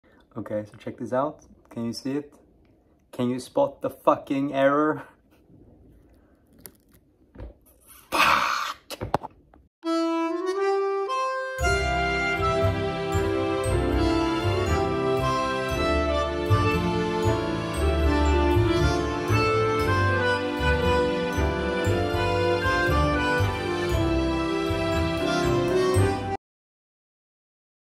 The Audio Jack from Hell
This is the 230th video I upload this year. and I felt good about this one. I have been cooking up some new coach potato videos lately and wanted to share them with you guys. This one we'll call a warm up though since I messed up the audio jack and recorded no audio.